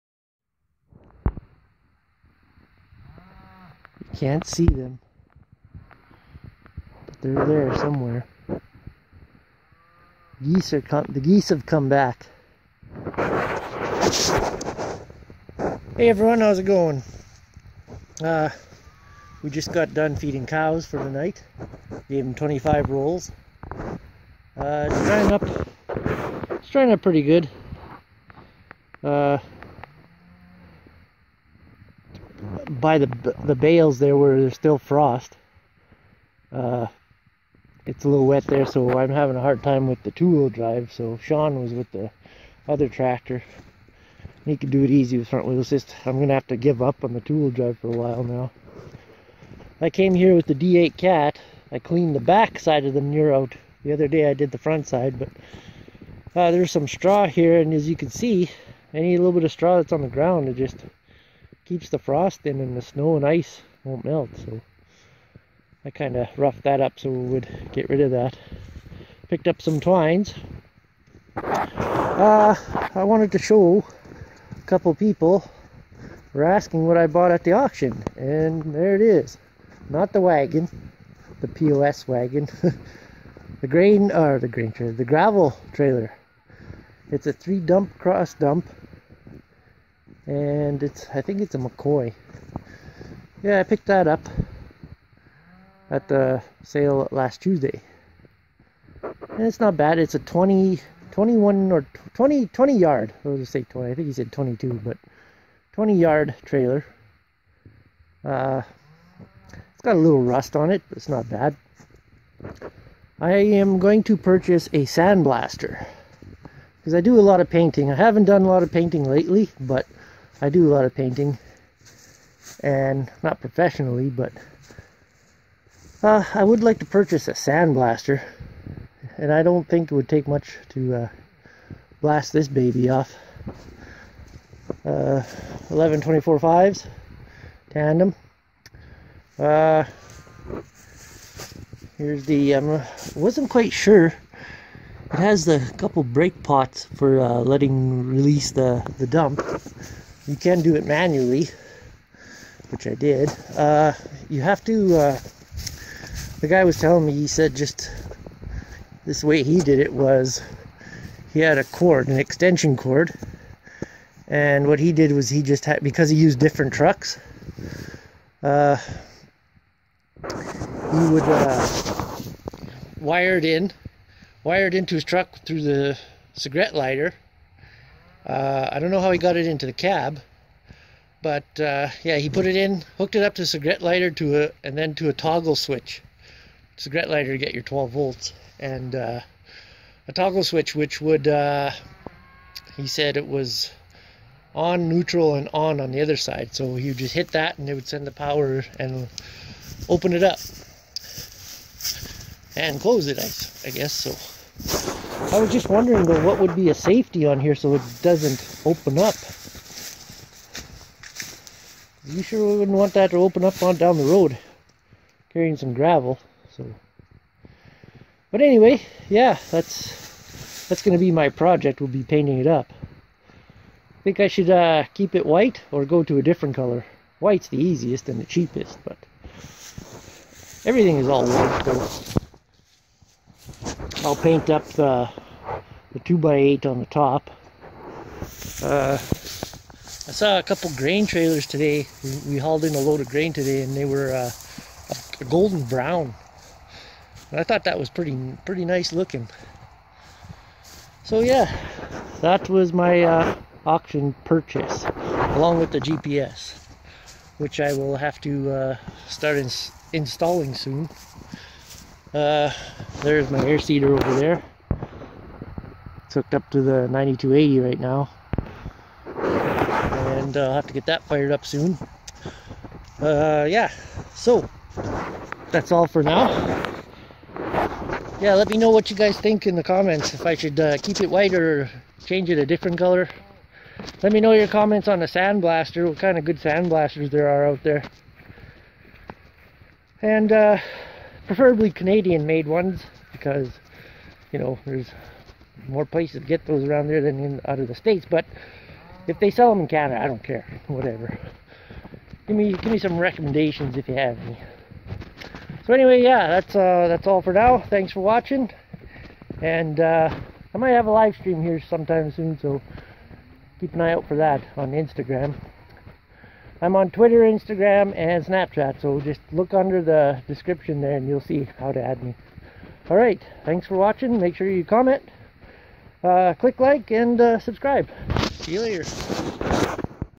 New Used Gravel Trailer
Thats what I bought at the auction. Cross dump gravel trailer.